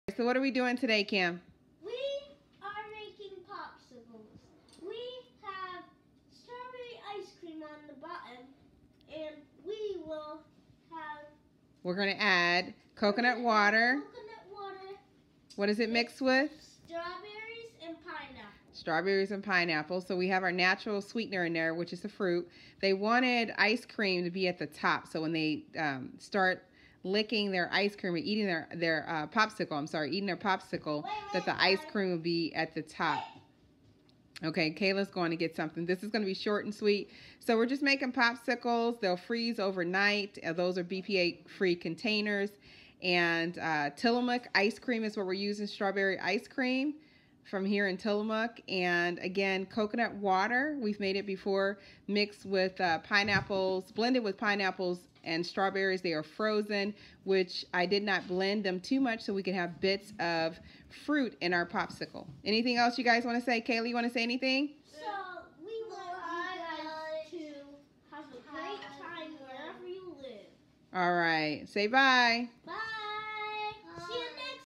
Homemade Popsicles and Transformation
Making your own popsicles at home can be fun and healthy.  Encourage our children to make healthier choices at a young age.